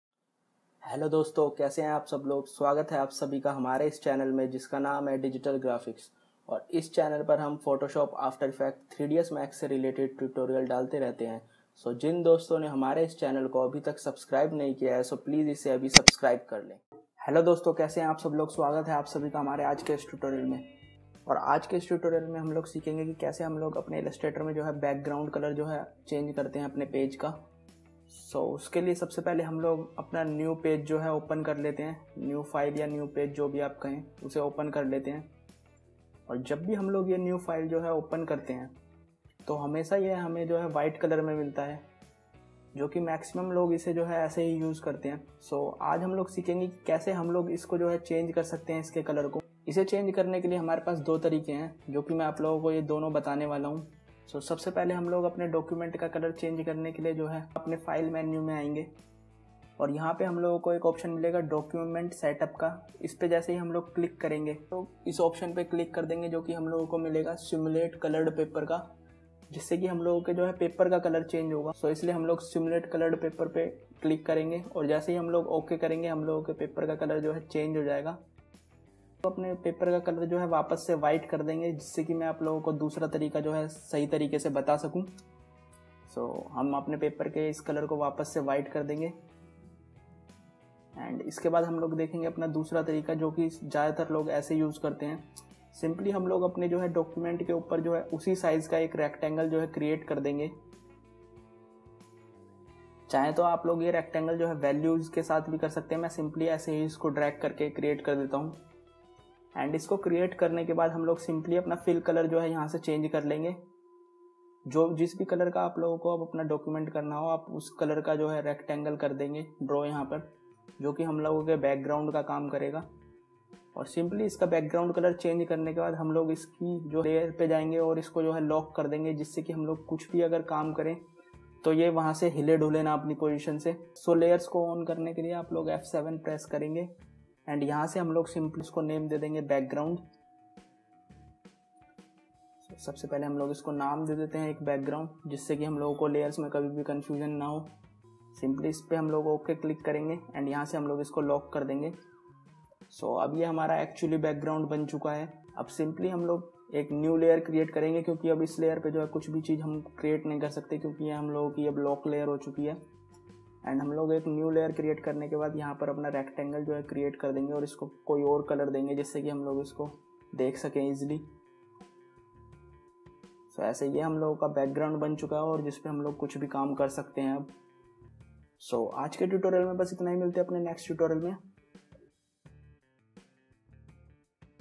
Adobe illustrator tutorial : How to change the background color in illustrator
In this tutorial of adobe illustrator we learn how to change the background color in illustrator and this is a Illustrator tutorial for beginners and it is a basic and important tutorial of illustrator and with the help of this you can change your background color and document or page color in illustrator and some people called this as artboard so how can we change the color